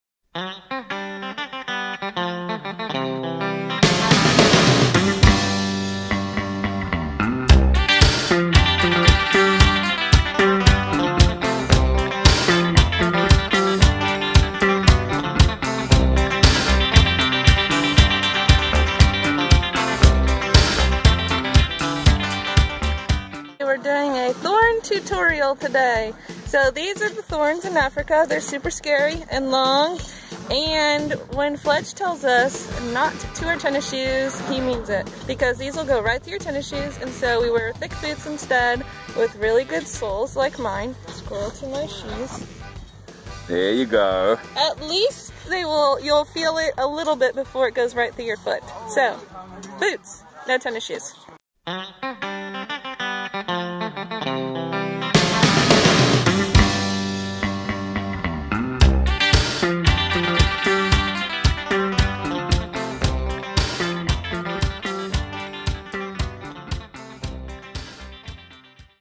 Son Safaris Mission Preparation - Why Do I Need Boots?
This video is about Why boots ae better than tennis shoes